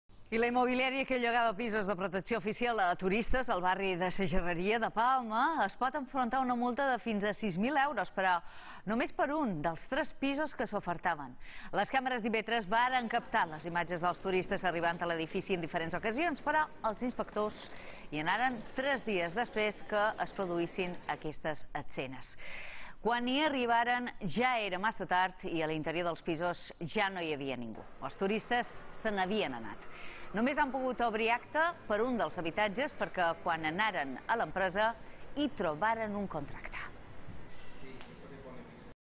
Turisme sanciona la immobiliària que llogava pisos de protecció oficial a turistes
La immobiliària que llogava pisos de protecció oficial a turistes al barri de sa Gerreria de Palma es pot enfrontar a una multa de fins a 6.000 euros però, només per un dels 3 pisos que s’ofertaven.  Les càmeres d’IB3 varen captar les imatges de turistes arribant a l’edifici en distintes ocasions, però els inspectors hi anaren tres dies després que es produïssin aquestes escenes. Quan hi arribaren ja era massa tard i a l’interior dels pisos ja no hi havia ningú. Els turistes se n’havien anat. Només han pogut obrir acta per un dels habitatges perquè, quan anaren a l’empresa, hi trobaren un contracte.